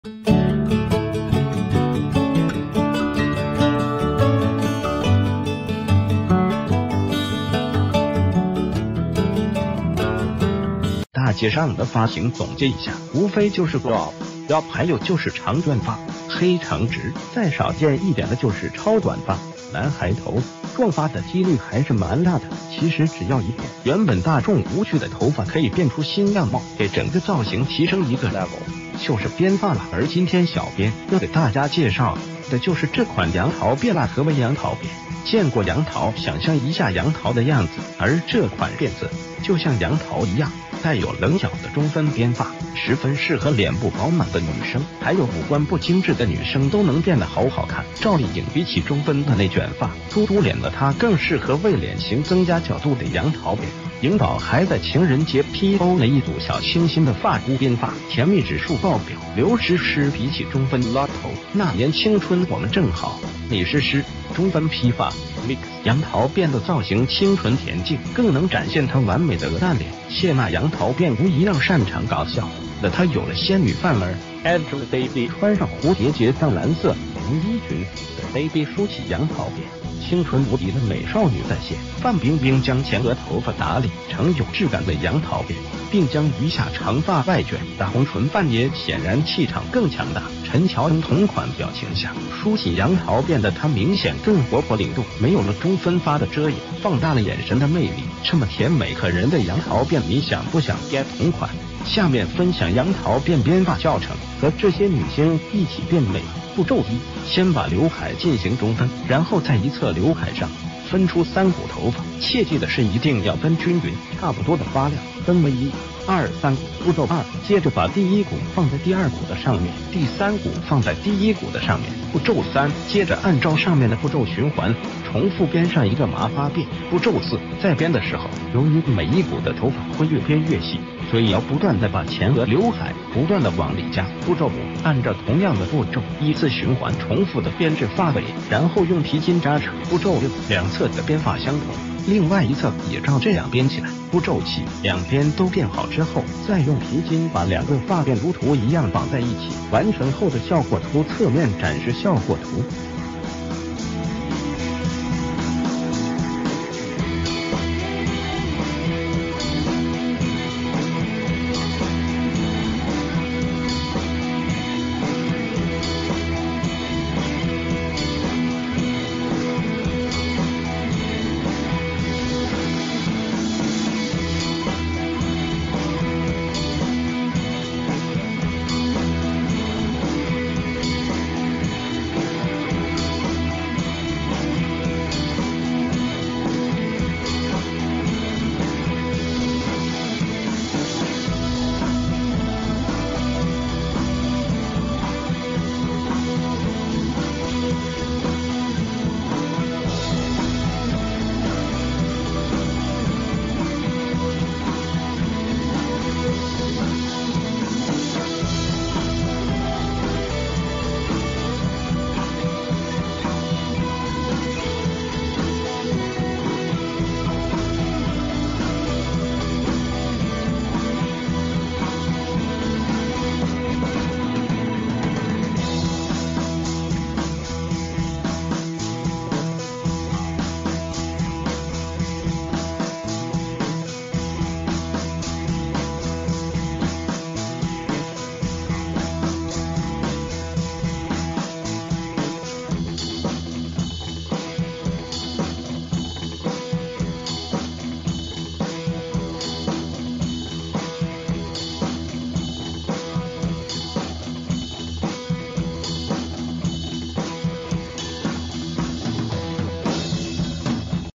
這款“楊桃辮”發型難度為0，顏值1000+，拐跑了全娛樂圈的女神！
大街上的發型總結一下，無非就是lob、bob，還有就是長卷發、黑長直，再少見一點的就是超短發、男孩頭……“撞發”的幾率還是蠻大的。
    其實只要一步，原本大眾無趣的頭發可以變出新樣貌，給整個造型提升一個level。
    就是編發啦
    而今天小編要給大家介紹的就是這款“楊桃辮啦”
    何為“楊桃辮”，見過楊桃想象一下楊桃的樣子，而這款辮子就像楊桃一樣帶有棱角的中分編發，十分適合臉部飽滿的女生，還有五官不精致的女生都能辮的好好看。
    趙麗穎
    比起中分的內卷發，嘟嘟臉的她更適合為臉型增加角度的“楊桃辮”。
    穎寶還在情人節PO了一組小清新的發箍編發，甜蜜指數爆表！
    劉詩詩
    比起中分LOB頭，《那年青春我們正好》里詩詩中分披發mix楊桃辮的造型清純恬靜，更能展現她完美的鵝蛋臉。
    謝 娜
    “楊桃辮”無疑讓擅長搞笑的她有了仙女范兒。
    Angelababy
    穿上蝴蝶結藏藍色連衣裙的Baby梳起“楊桃辮”，青春無敵的美少女再現。
    范冰冰
    將前額頭發打理成有質感的“楊桃辮”，并將余下長發外卷的紅唇范爺顯然氣場更強大。
    陳喬恩
    同款表情下，梳起“楊桃辮”的她明顯更活潑靈動，沒有了中分發的遮掩，放大了眼神的魅力。
    這么甜美可人的楊桃辮你想不想get同款?
    下面分享楊桃辮編發教程，和這些女星一起變美吧~
    步驟一：先把劉海進行中分，然后在一側劉海上分出3股頭發。切記的是一定要分均勻，差不多的發量，分為1、2、3股。
    步驟二：接著把第一股放在第二股的上面，第三股放在第一股的上面。
    步驟三：接著按照上面的步驟循環重復編上一個麻花辮。
    步驟四：在編的時候，由于每一股的頭發會越編越細，所以要不斷的把前額劉海不斷的往里加。
    步驟五：按照同樣的步驟，依次循環重復的編至發尾，然后用皮筋扎上。
    步驟六：兩側的編法相同，另外一側也照這樣編起來。
    步驟七：兩邊都變好之后，再用皮筋把兩個發辮如圖一樣綁在一起。
    完成后的效果圖
    側面展示效果圖